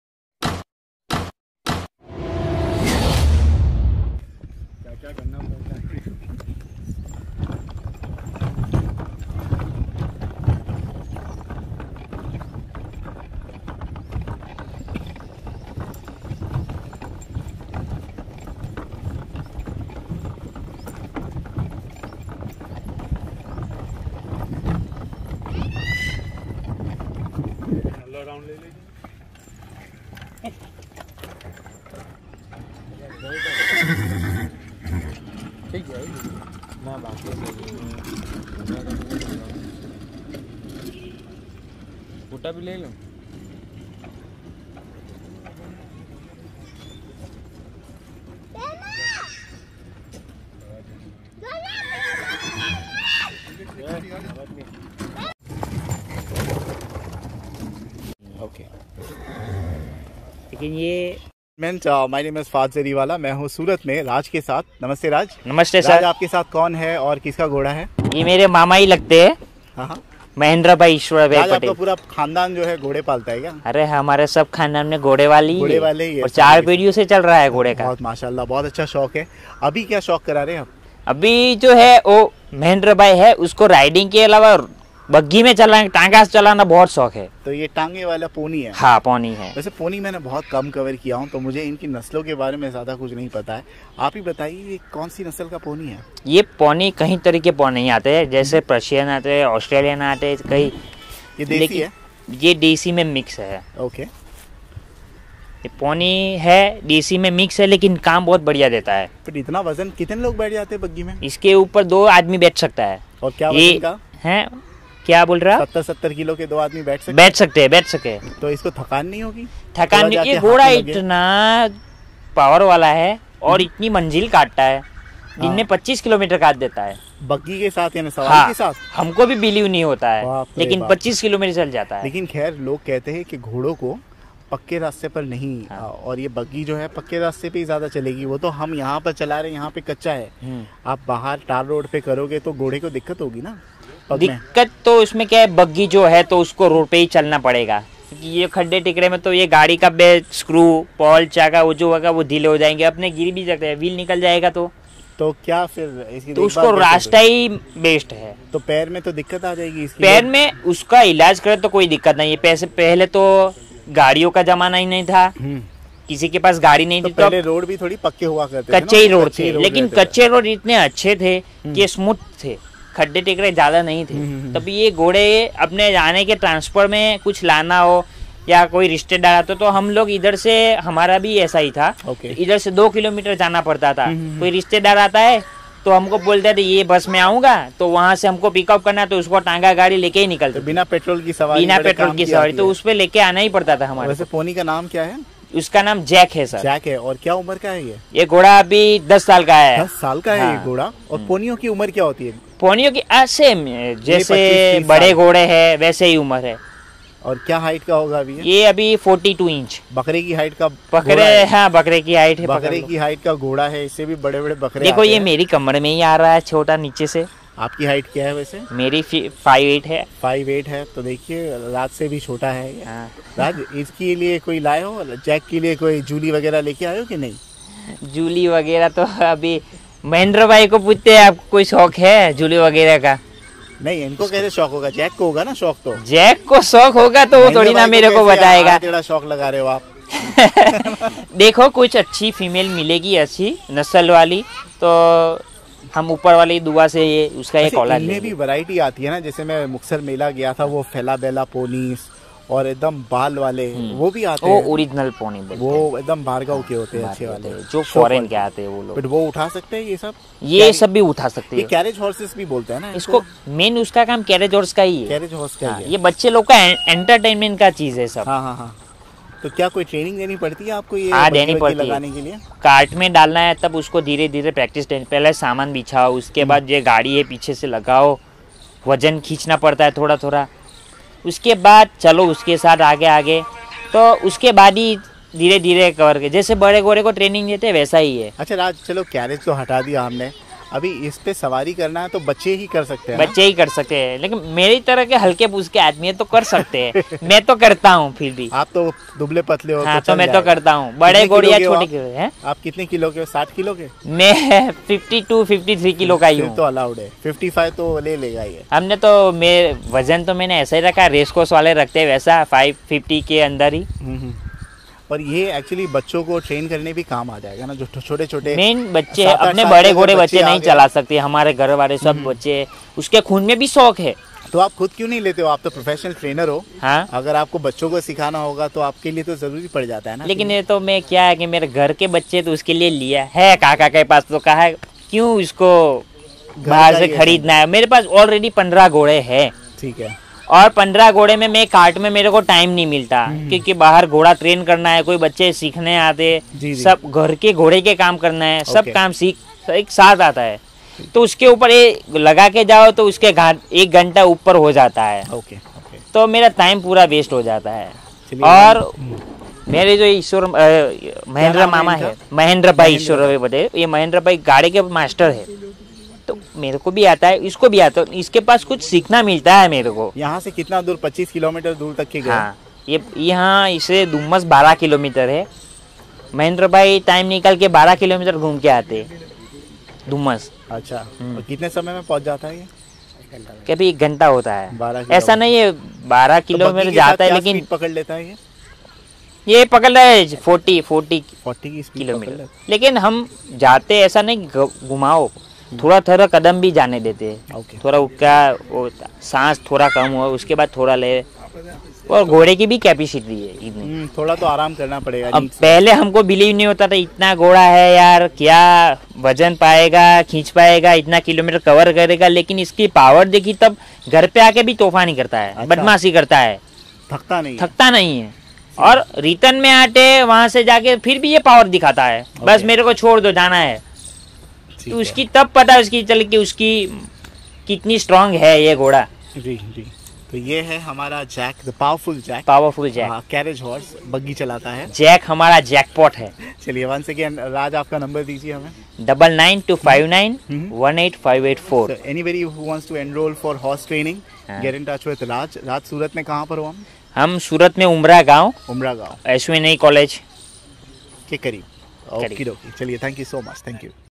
Horse Buggy Ride With Pony Jack at Hasu Bhai Patel's Stud Farm Surat
Raj is a horse trainer and to enroll for horse riding training Plz contact Raj at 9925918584